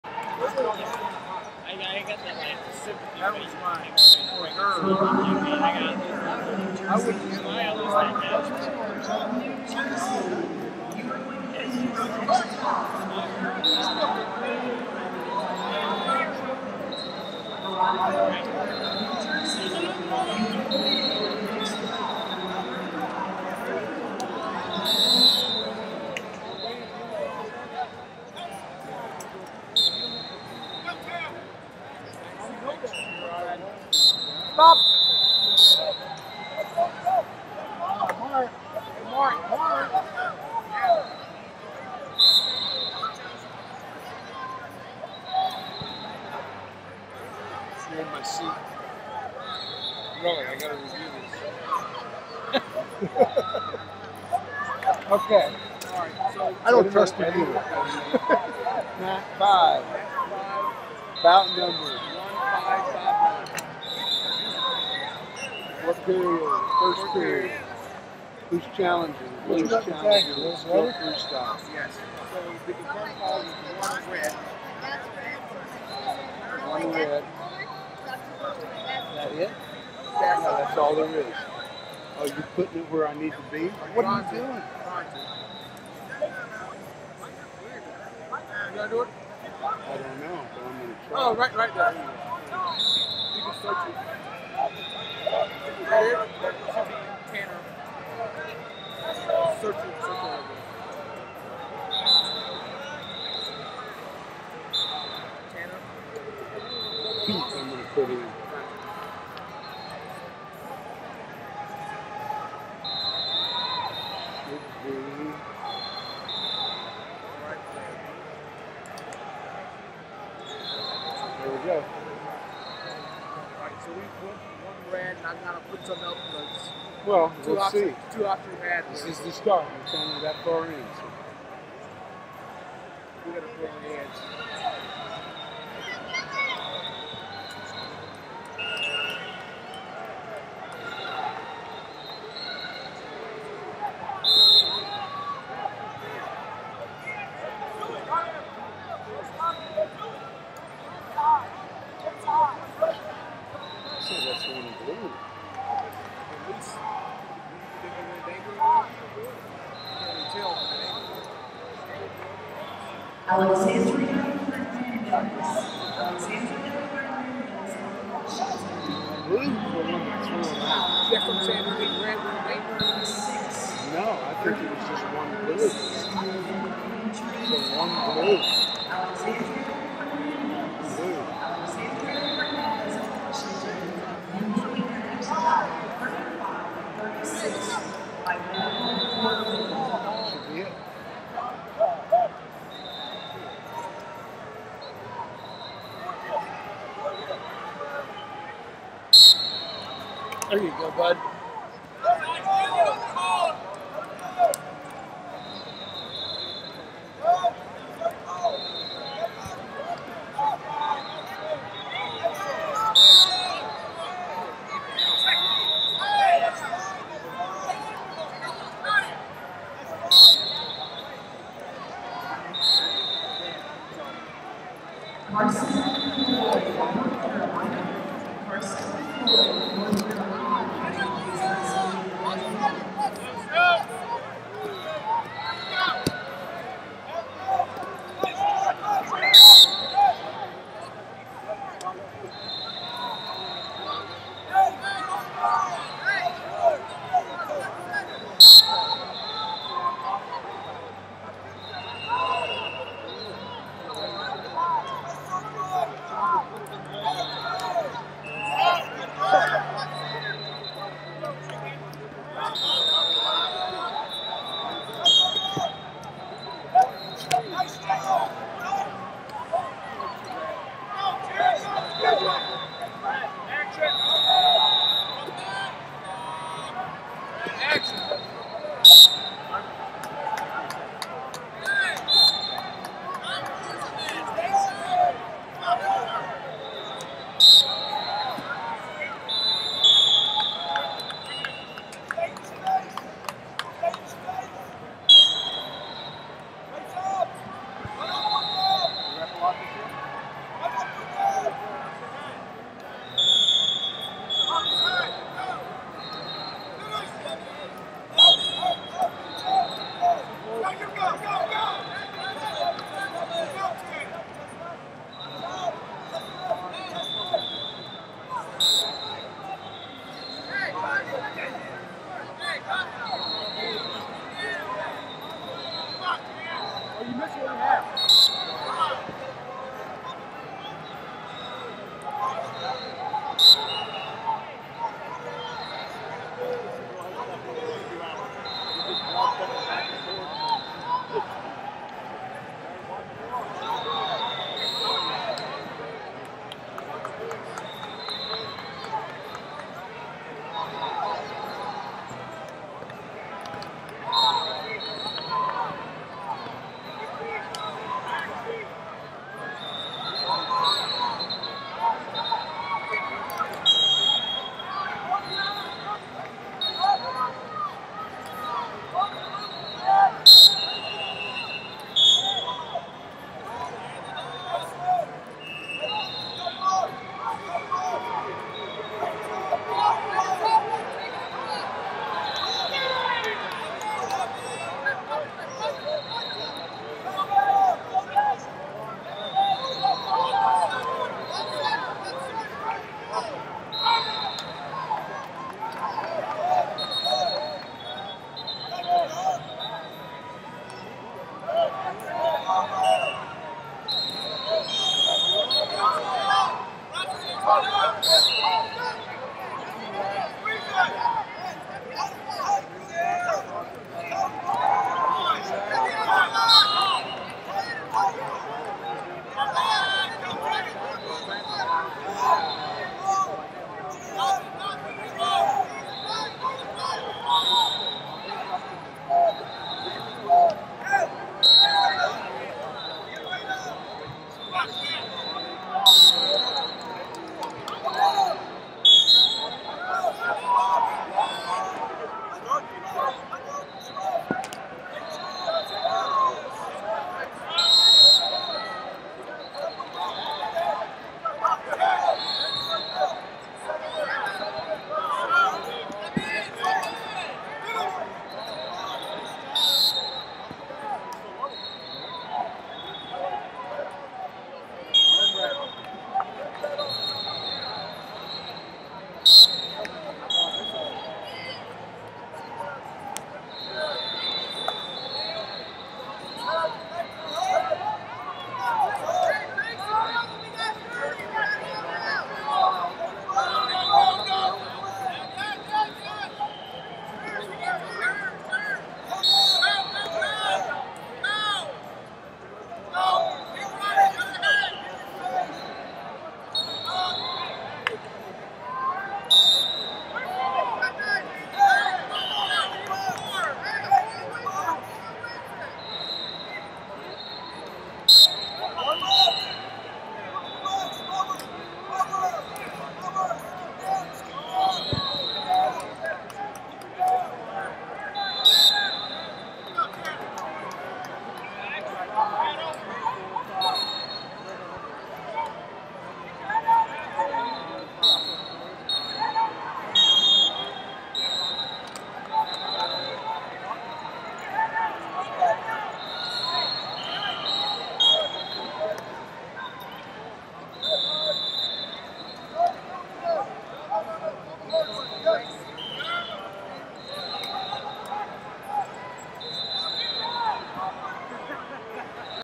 138 Lbs Cons 32 2 Tanner Peake New Jersey Vs Ben Richardson Connecticut
2019 USMC USAW Junior And Cadet National Championships - 138 Lbs Cons 32 2 Tanner Peake New Jersey Vs Ben Richardson Connecticut